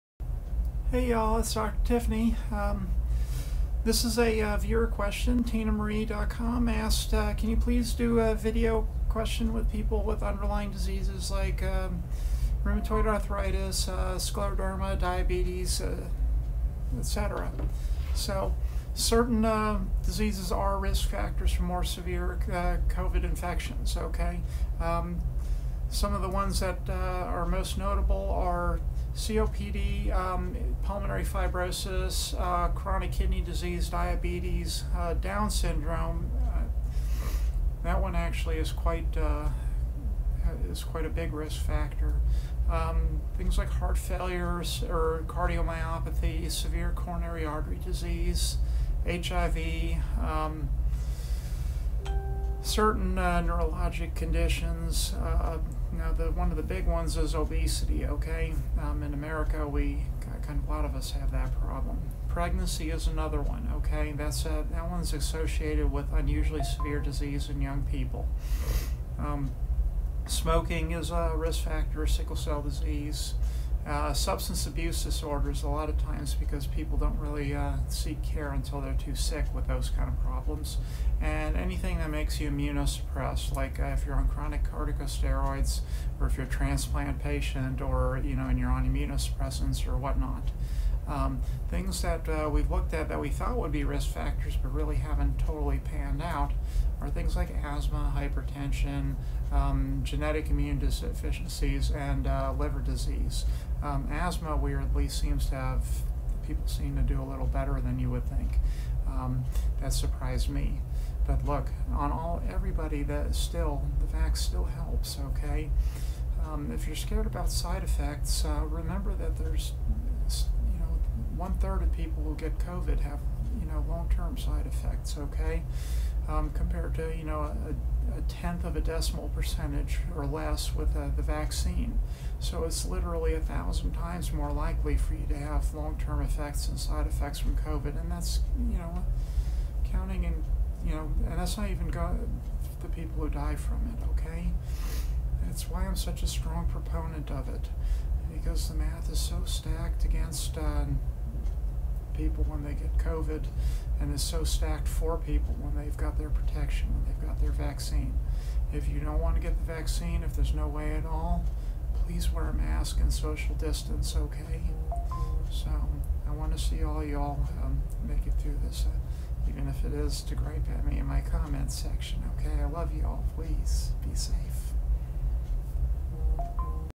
Underlying diseases and covid
I explore diseases that increase the risk of severe covid